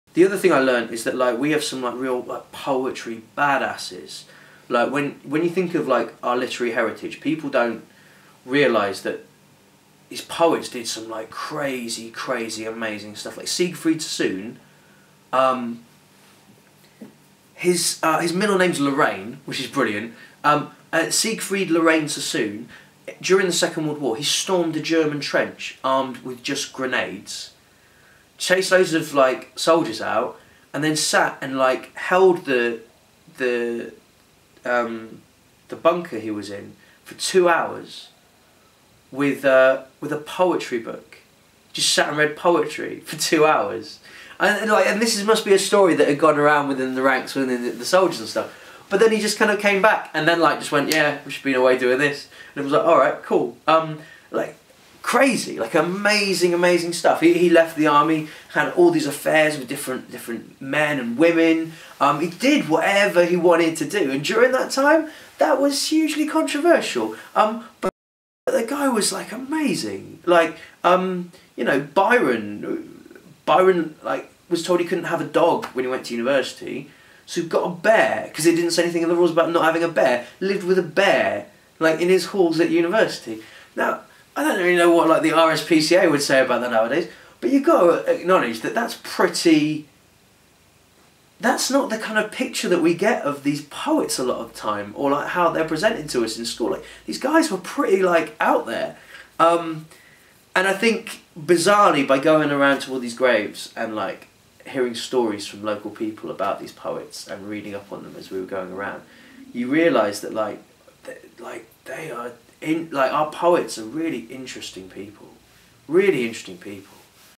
Mark Grist on 'badass poets'
Poet Mark Grist talks about his experience on his tour of British Poets' graves and how poets are not the boring people that many may think they are...